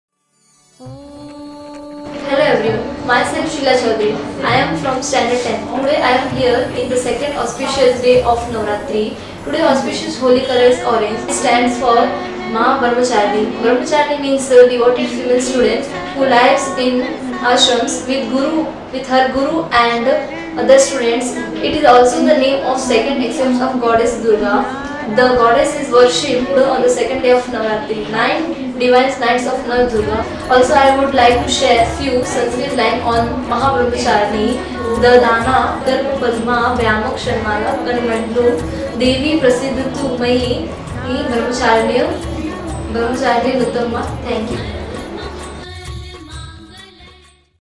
NAC Navratri Special - Orange (Ft. Sheela)
As this Event comes only ones a year, Our student prepare a short speech representing every form of Devi with her Dress code showing her's Identity!🇮🇳💜🔥